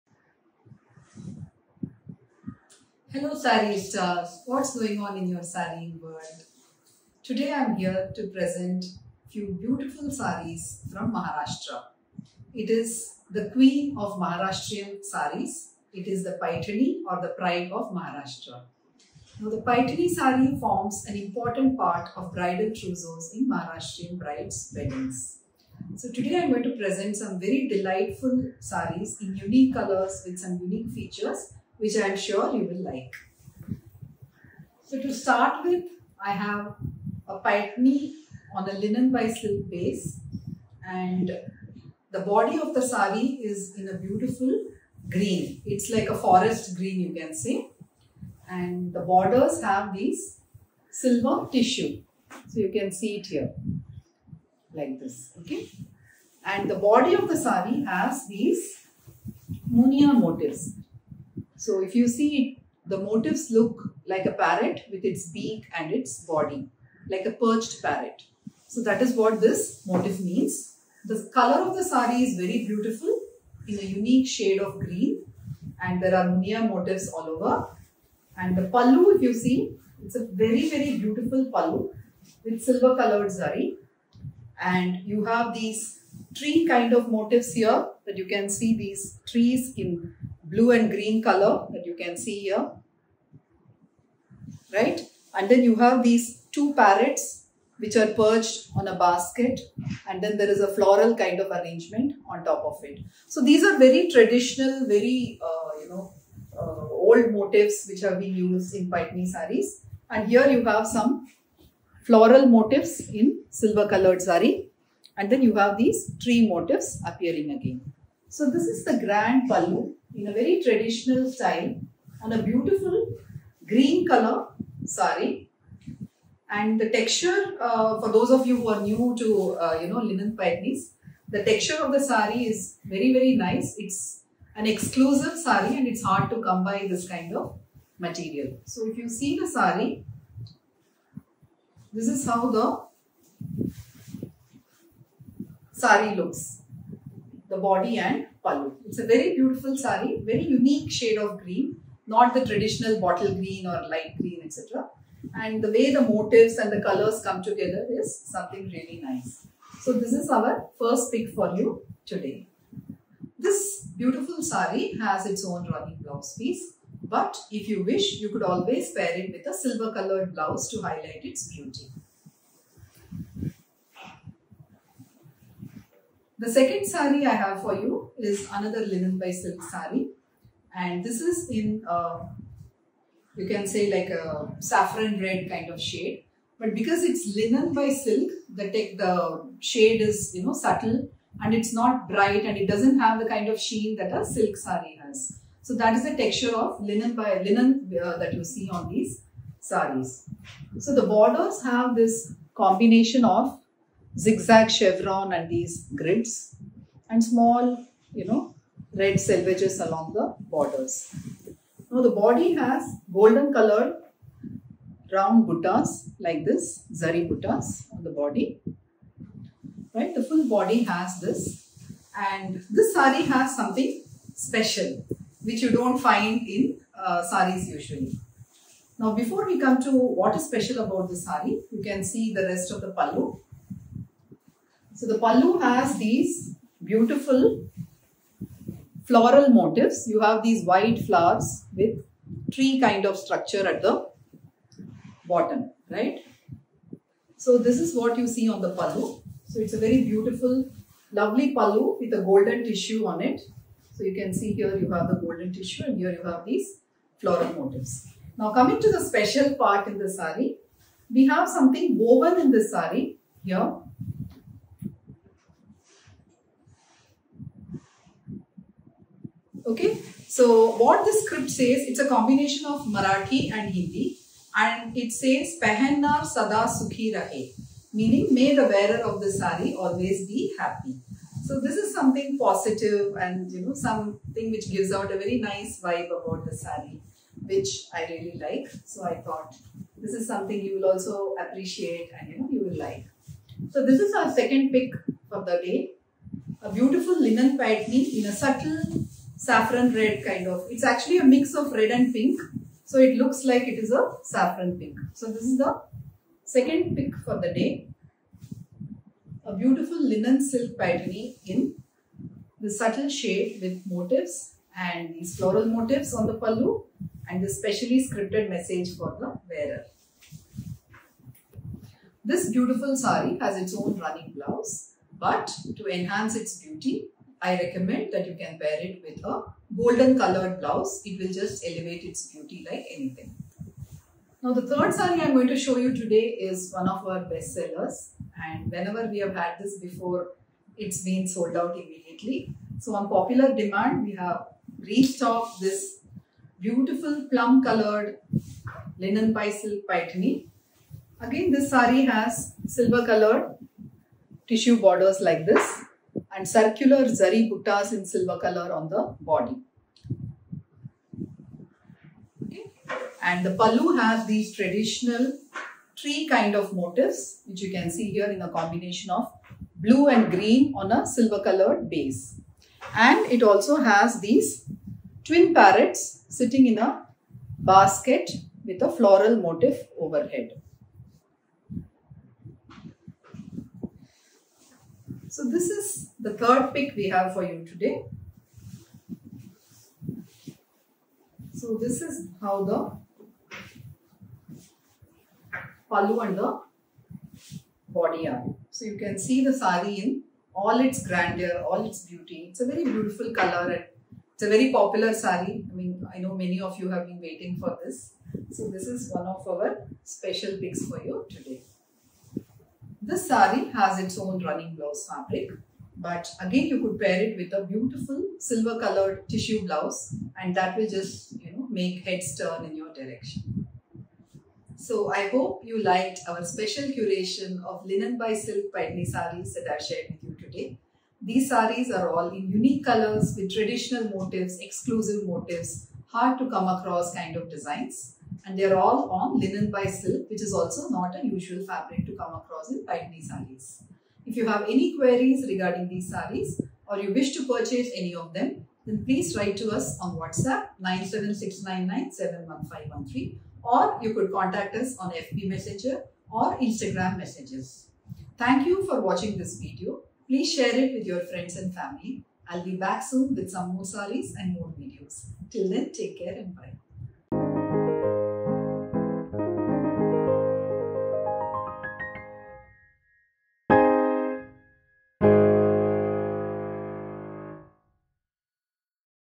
Paithani sarees from Maharashtra made in exclusive designs on linen silk
Presenting special and exclusive linen silk Paithanis in unique colors, perfect for this wedding season!